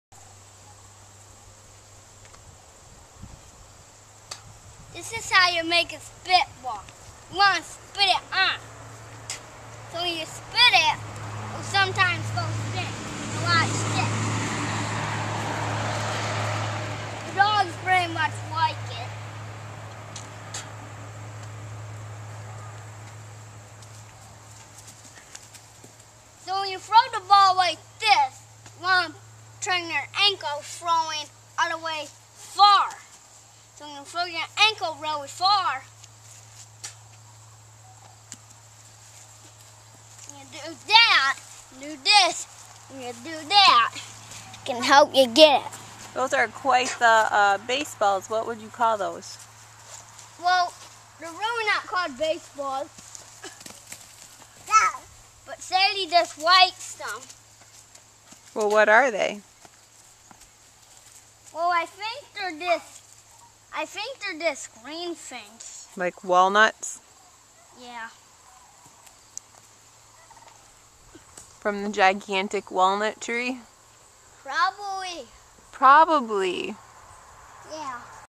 Walnut Baseball
Short on baseballs?  Learn to play ball the country boy way, with walnuts.